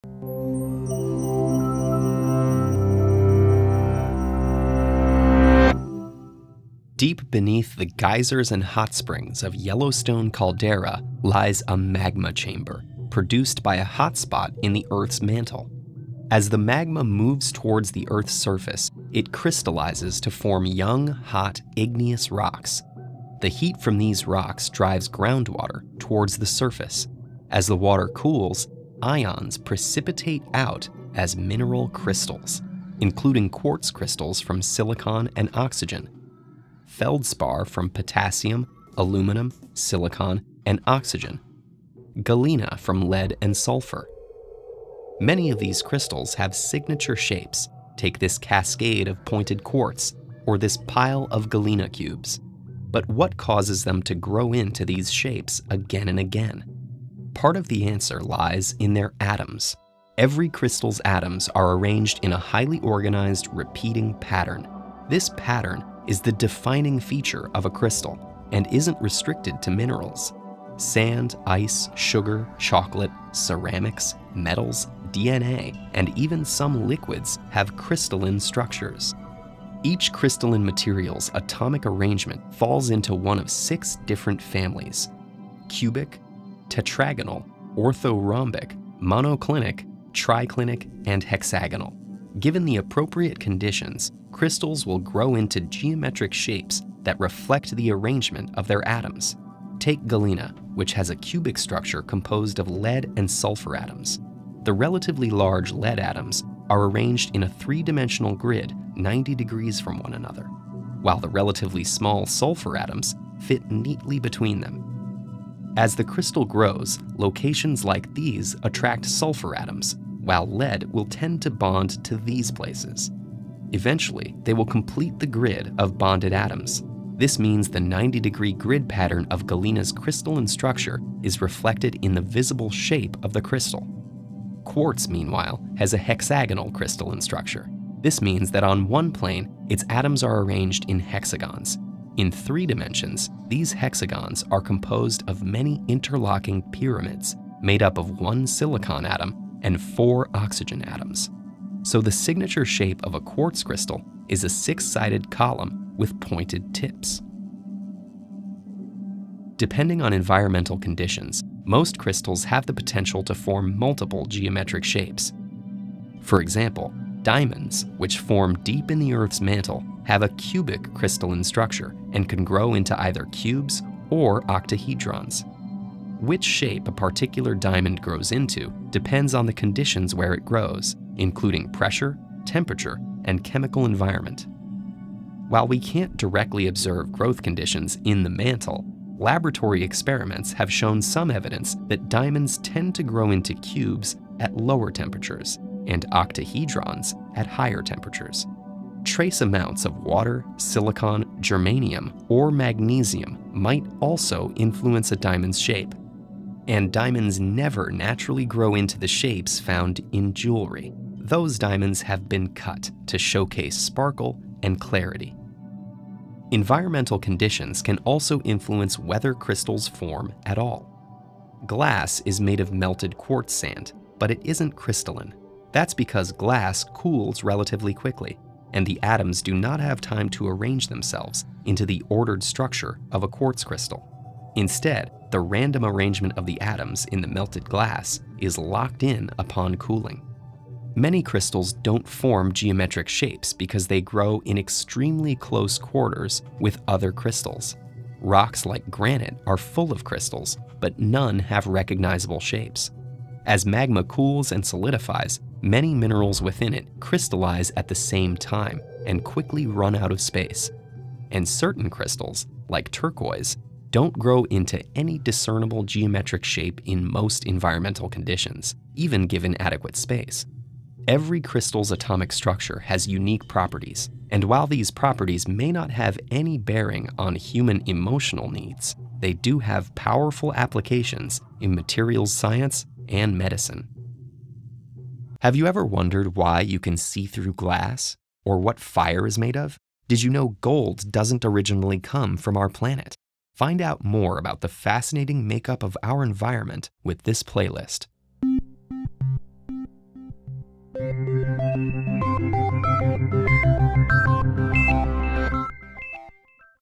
How do crystals work? - Graham Baird
What makes crystals grow into their signature shapes? Dig into the atomic patterns and unique properties of crystals.

Many crystals have signature shapes— like the cascade of pointed quartz or a pile of galena cubes. Every crystal’s atoms have a defining feature: their organized, repeating pattern. The pattern isn't restricted to minerals- sand, ice, metals and DNA also have crystalline structures. So what causes them to grow into these shapes again and again? Graham Baird dives into the unique properties of crystals.

Lesson by Graham Baird, directed by Franz Palomares.

Thank you so much to our patrons for your support! Without you this video would not be possible!